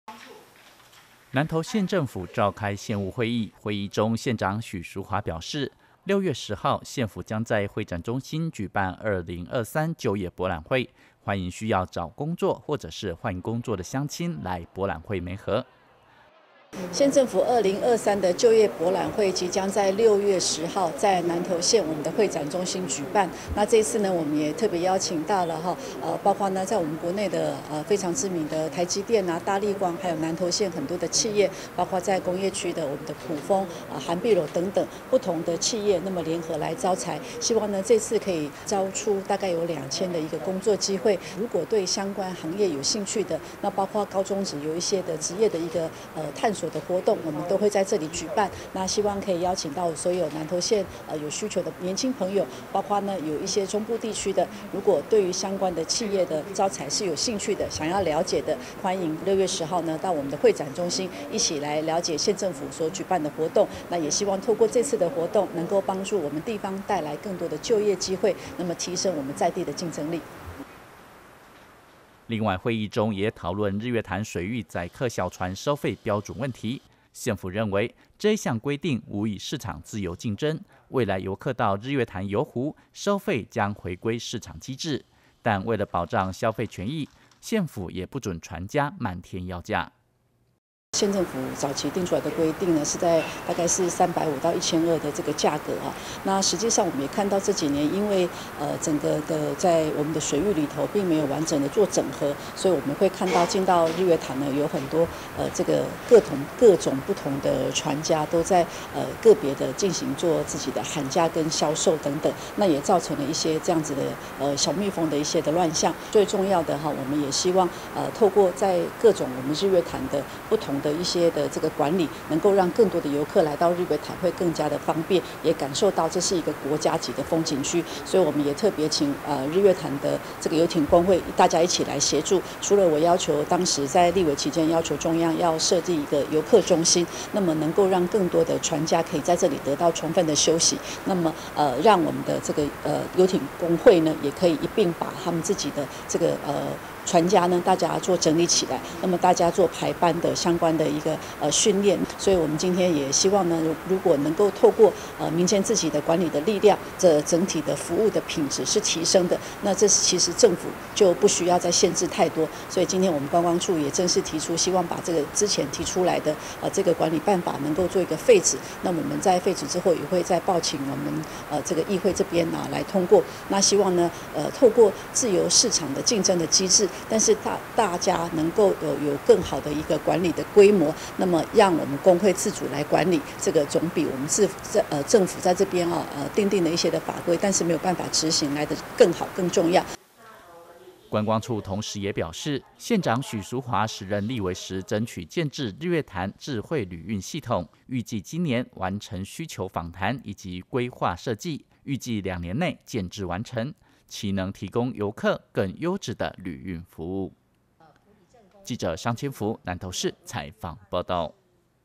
2023就業博覽會 6月10日會展中心隆重登場｜南投縣政新聞 2023.06.05
南投縣政府5日召開縣務會議中，縣長許淑華表示，6月10日縣府舉辦就業博覽會活動，歡迎需要找工作或換工作的鄉親，來博覽會媒合，另外有關日月潭載客小船收費標準問題，縣府認為，這項規定無益市場自由競爭，未來遊客到日月潭遊湖，收費將回歸市場機制，但為保障消費權益，縣府也不准船家漫天要價。